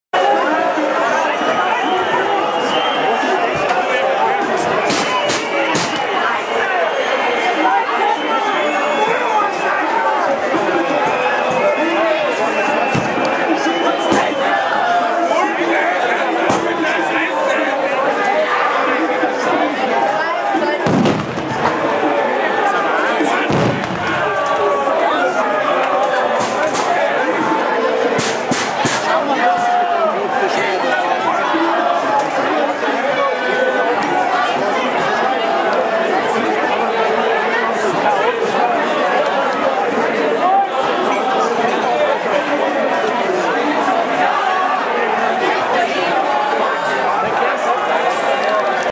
GAK Fans Platzsturm in Hartberg , Relegationsspiel, 08.06.2012 (1080p / HD)
Mit solchen Fans kannst dich verabschieden. Glücklicherweise sind sie eine Minderheit, leider erziehlen sie großen Schaden. Das Video soll dem GAK helfen diese Minderheit loszuwerden.